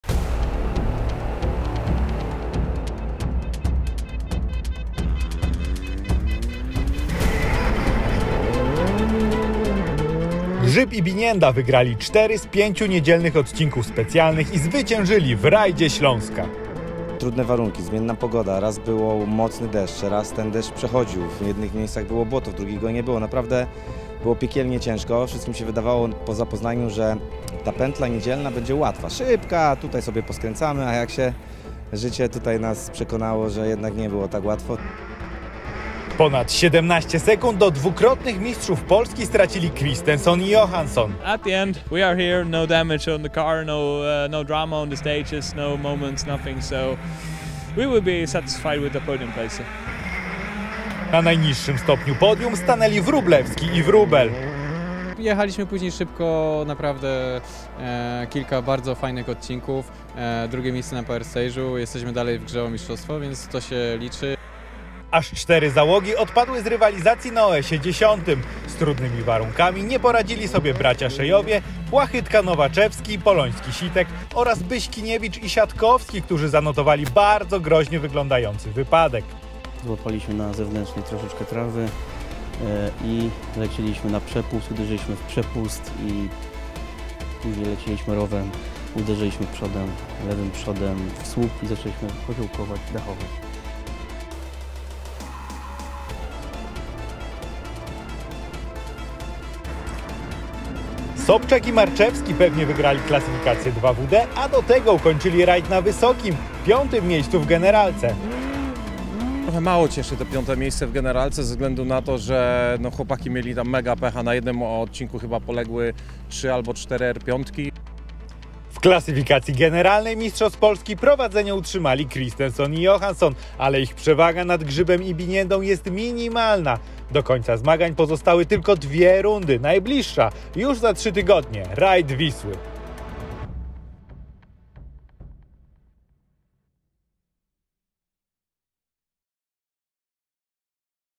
Rajd Śląska 2022: podsumowanie II etapu
Rajd Śląska 2022 to szósta runda sezonu 2022 Rajdowych Samochodowych Mistrzostw Polski.